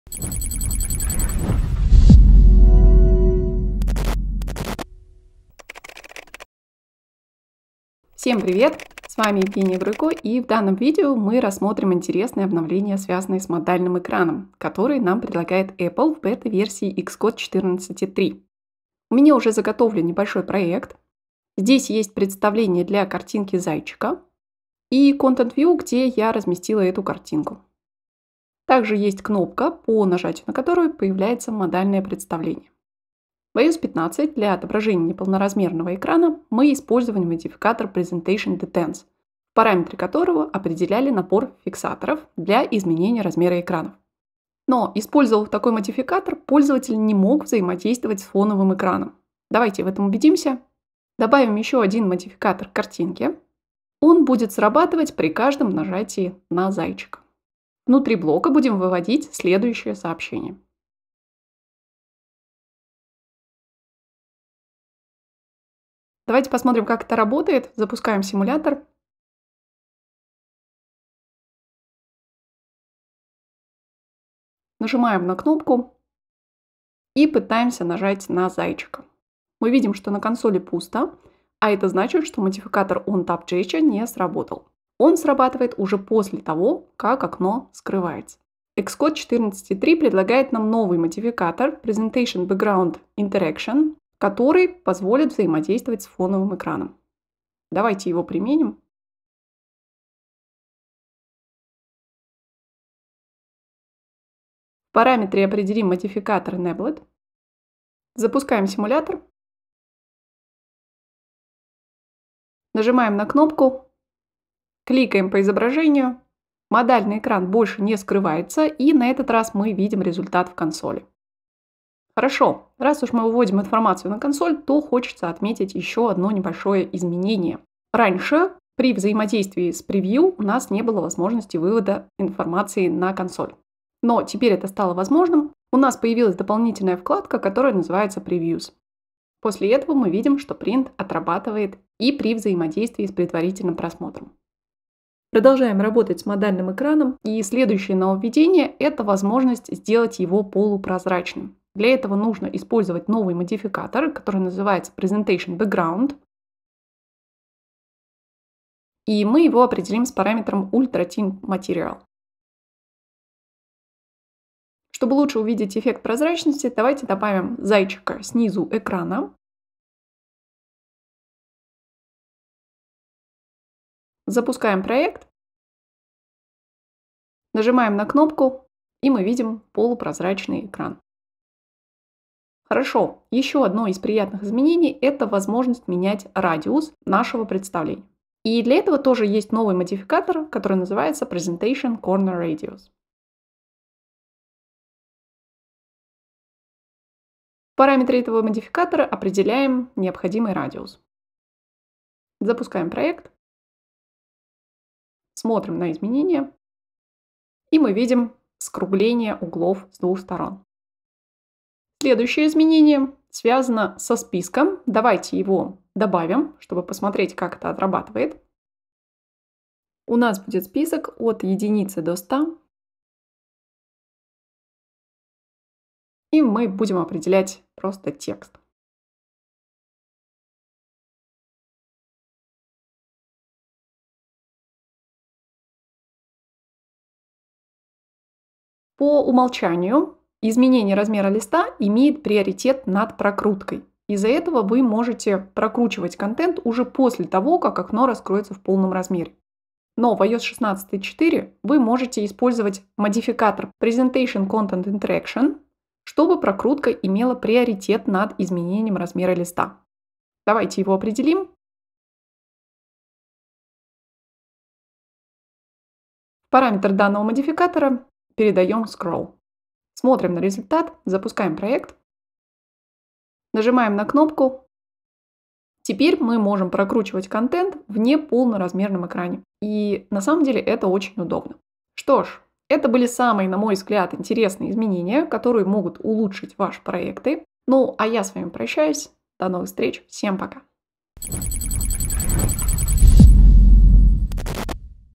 Что нового в Xcode 14.3 beta и iOS 16.4
Всем привет!

В данном видео мы рассмотрим интересные обновления, связанные с модальным экраном, которые нам предлагает Apple в бета версии  xcode 14.3.

Поехали!

Подробнее о нашем курсе "Профессия iOS-разработчик", вы можете узнать на нашем сайте